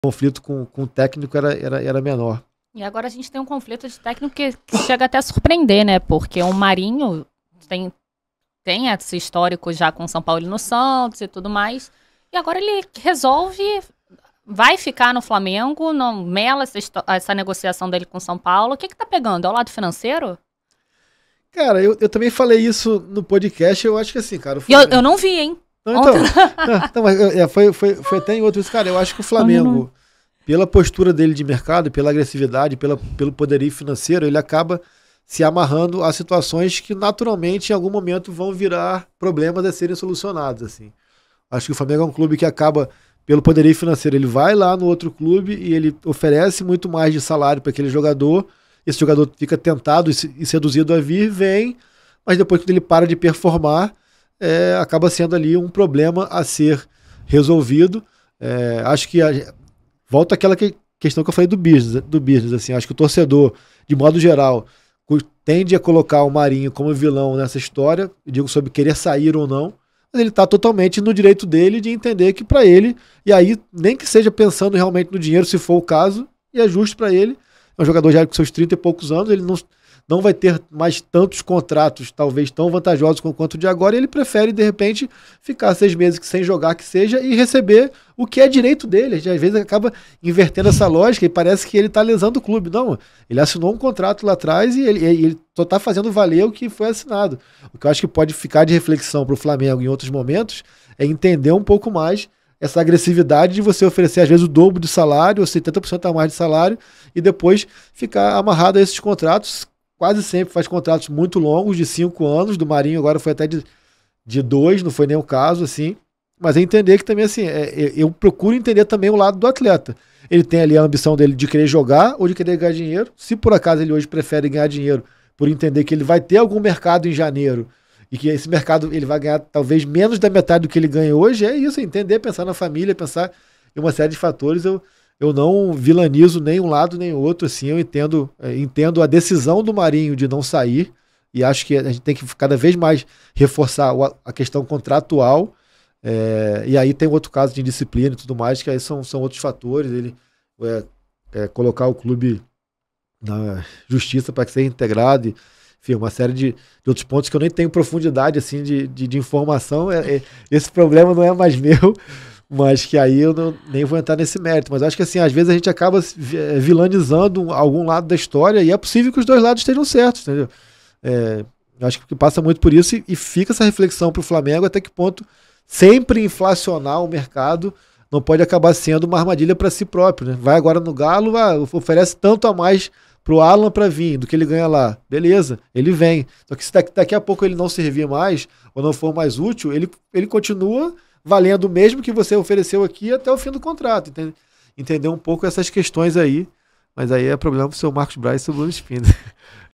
A SITUAÇÃO DO MARINHO NO FLAMENGO!
O Que Partidazo #6 recebe o jornalista Cahê Mota.

Para assistir a resenha completa com o Cahê Mota é só clicar no link abaixo: